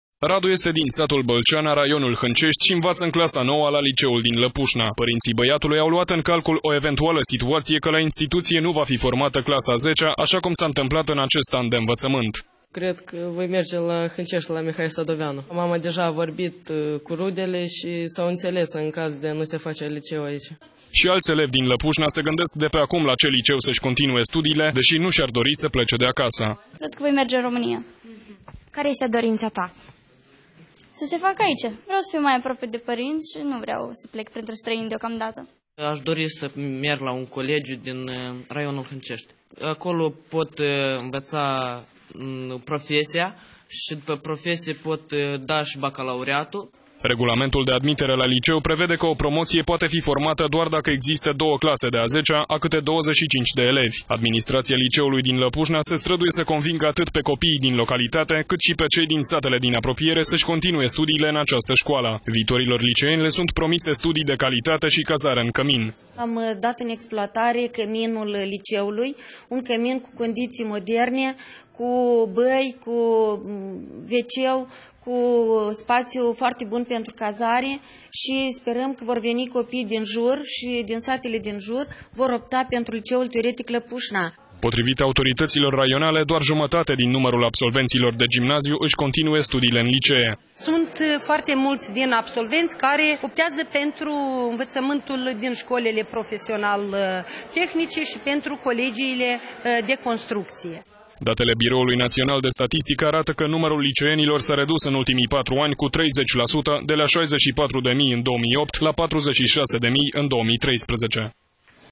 Republica Moldova licee pustii
Datele Biroului Naţional de Statistică arată că numărul liceenilor s-a redus în ultimii patru ani cu 30 la sută, de la 64 de mii în 2008, la 46 de mii în 2013.